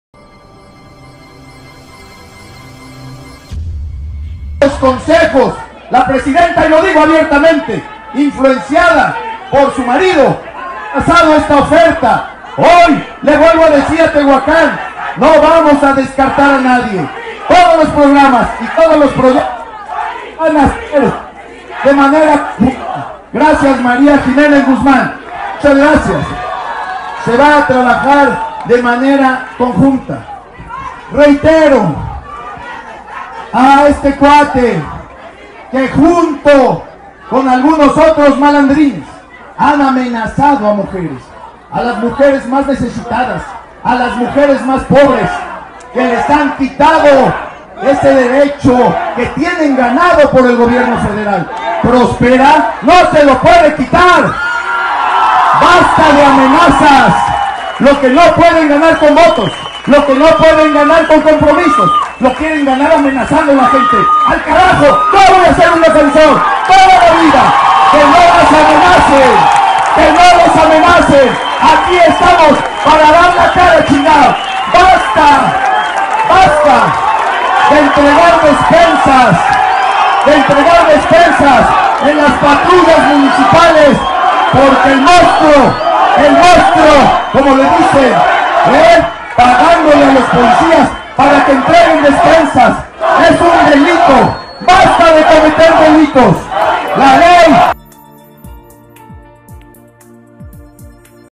Gali último mitin en Tehuacán
A unas horas de que concluya la etapa de proselitista de campaña el candidato de la Coalición Sigamos Avanzando, Antonio Gali Fayad visitó el municipio de Tehuacan en donde se comprometió a construir el bulevar principal y a trabajar de manera conjunta con las autoridades municipales.